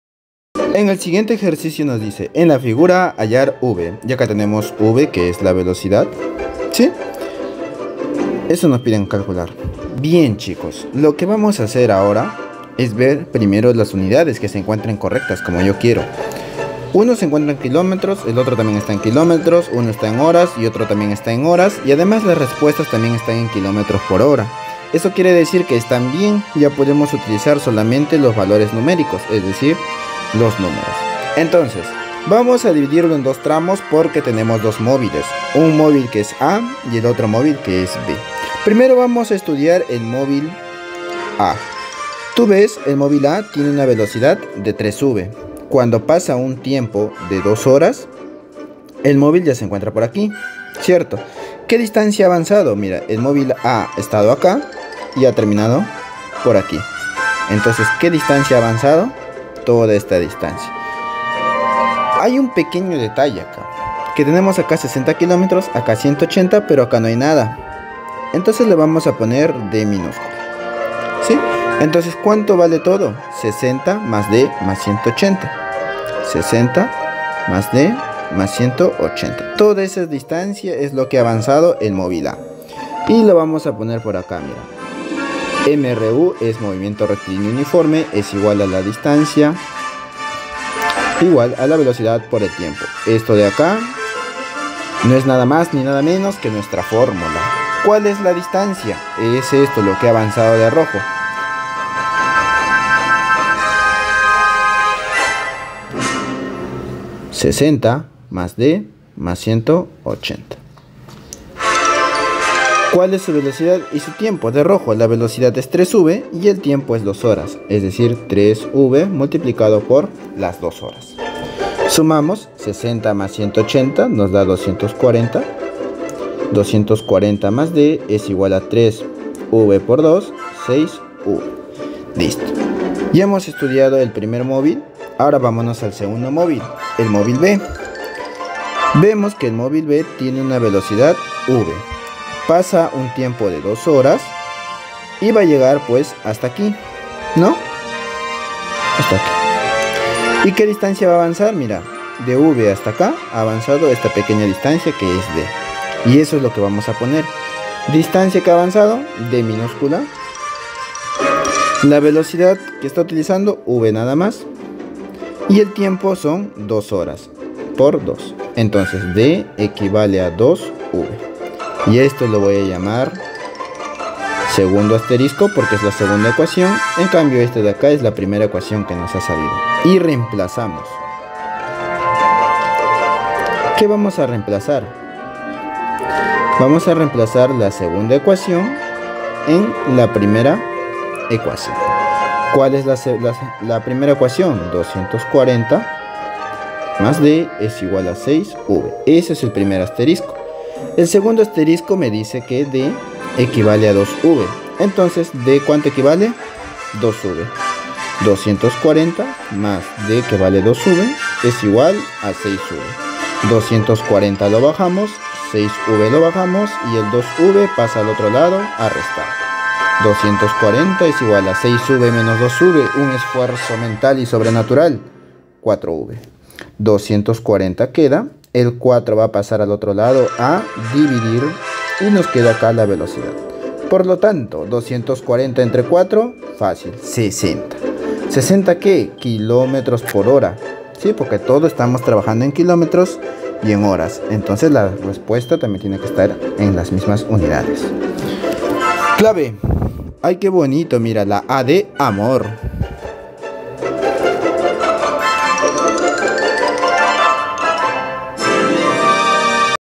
En la figura, hallar: “V” Tiempo 2 horas. velocidades 3V y V. distancias 60km y 180km
TEMA: MOVIMIENTO RECTILÍNEO UNIFORME 2 - MRU II
CURSO: FÍSICA
Ejercicios resueltos de TERCERO de Secundaria 3° sec
En la figura, hallar : “V”
a) 60 km/h
b) 180
c) 20
d) 30
e) 45
Tiempo 2 horas.
velocidades 3V y V.